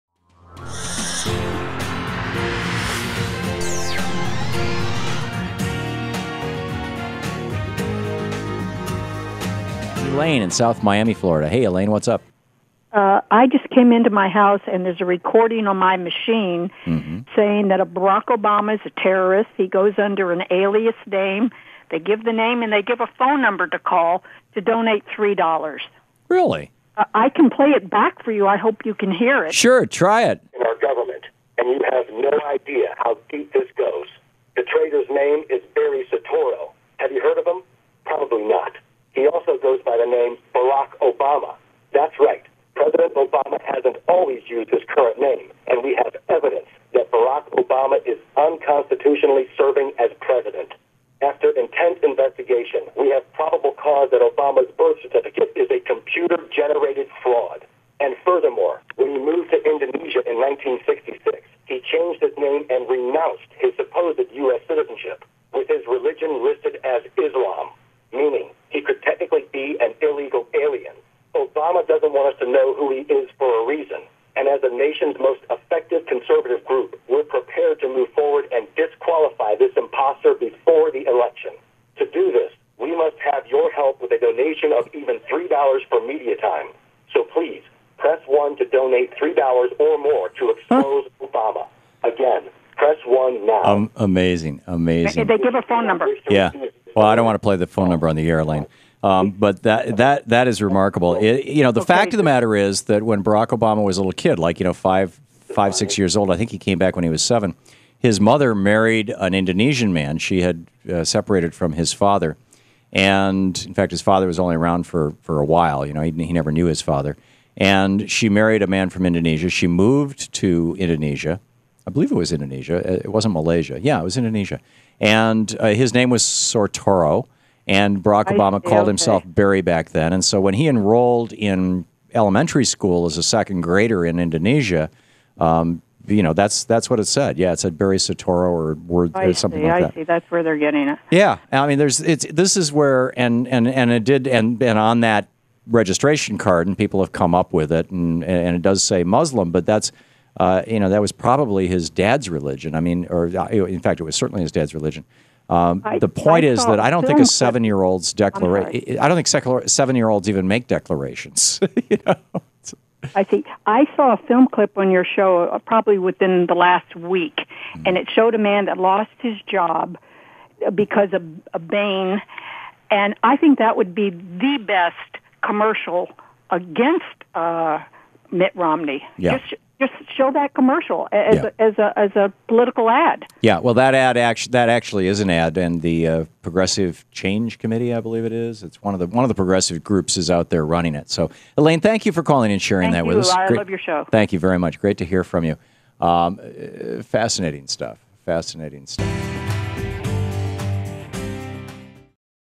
Callers Anti-Obama Robocall
A caller from Florida shares a robo call message left on her answering machine that claims President Obama is not who he says he is.